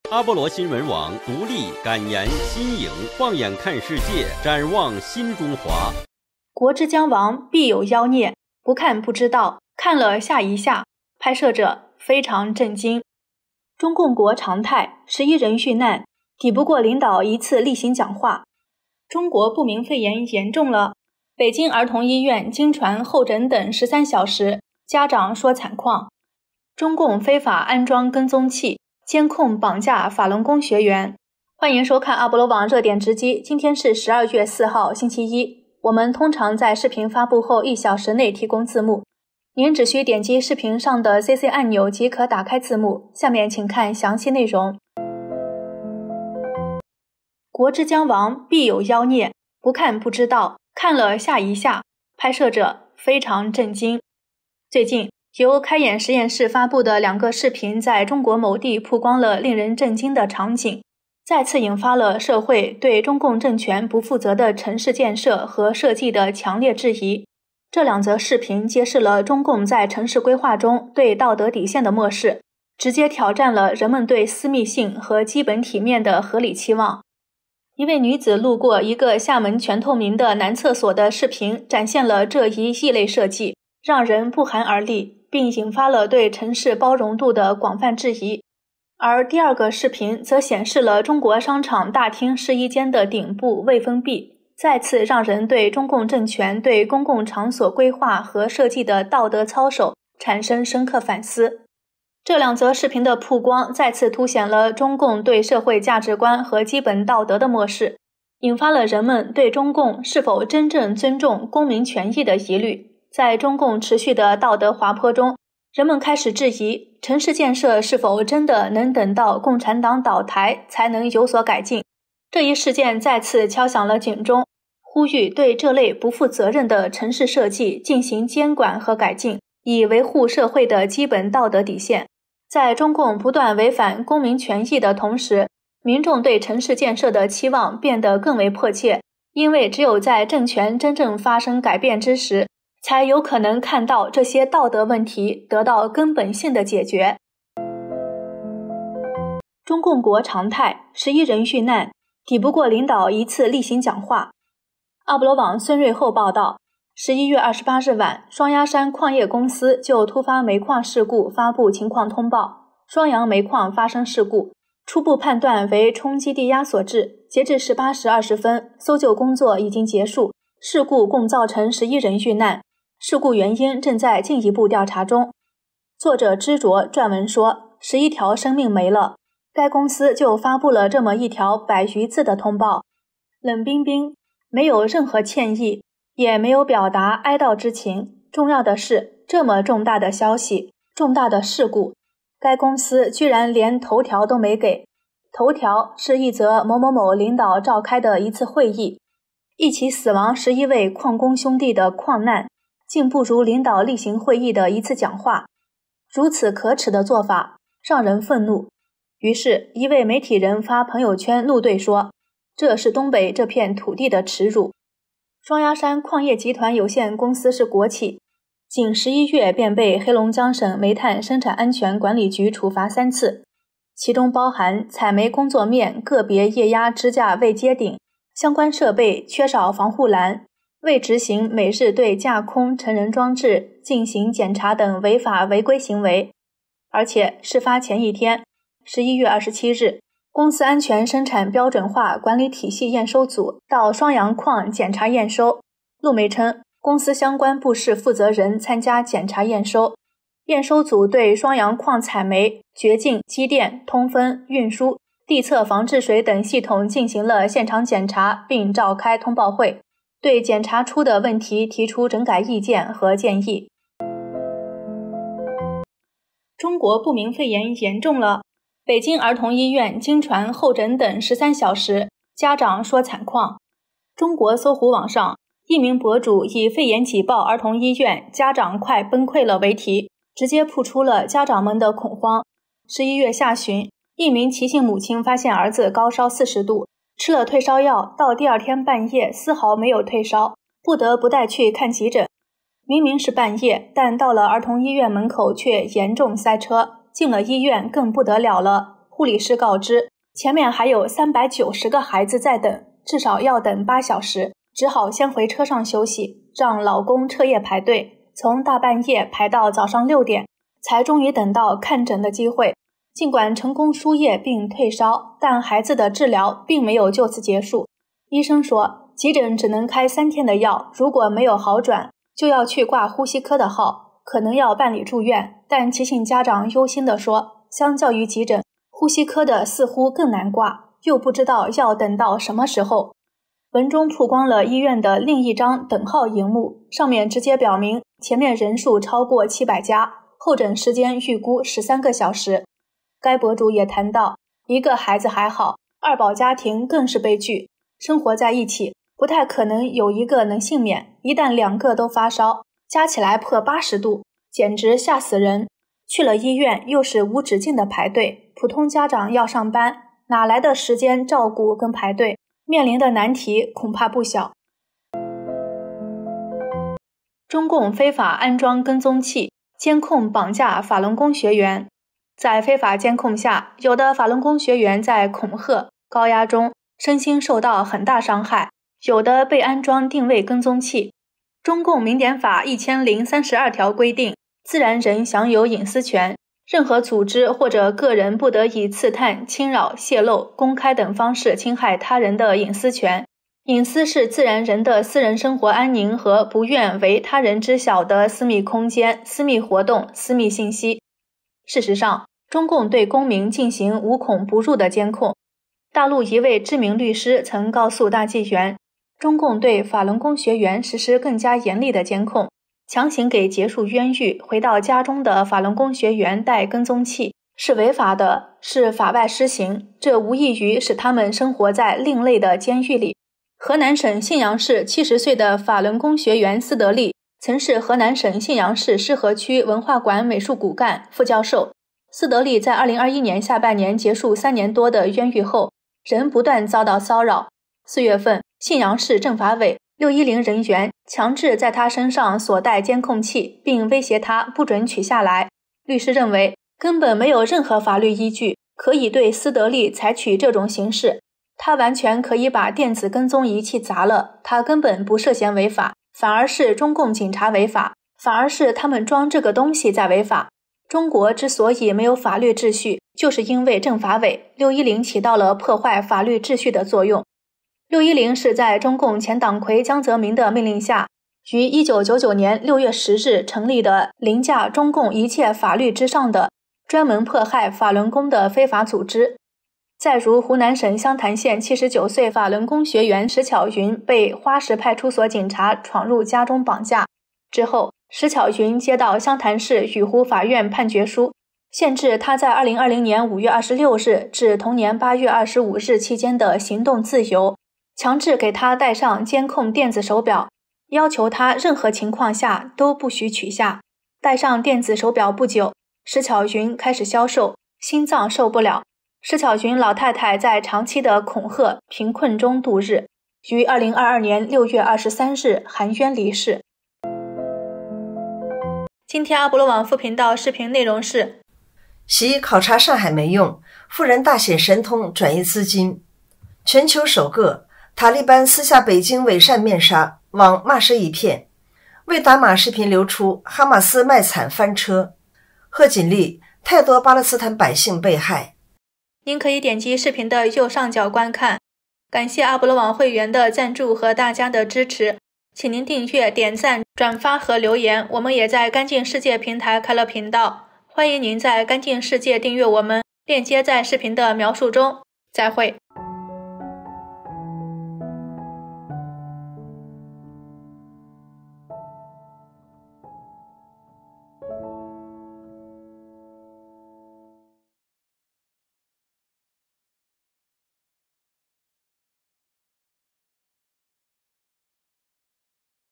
🔥國之將亡必有妖孽：不看不知道 拍攝者：非常震驚！中國不明肺炎嚴重了？北京醫院驚傳候診等13小時家長說慘況；中共國常態：11人遇難，抵不過領導講話；中共非法安裝跟蹤器 監控綁架法輪功學員【阿波羅網C】
00:00 摘要
00:46 國之將亡必有妖孽：不看不知道 看了嚇一下 拍攝者：非常震驚！
02:29 中共國常態：11人遇難，抵不過領導一次例行講話
04:25 中國不明肺炎嚴重了？北京兒童醫院驚傳候診等13小時家長說慘況
06:06 中共非法安裝跟蹤器 監控綁架法輪功學員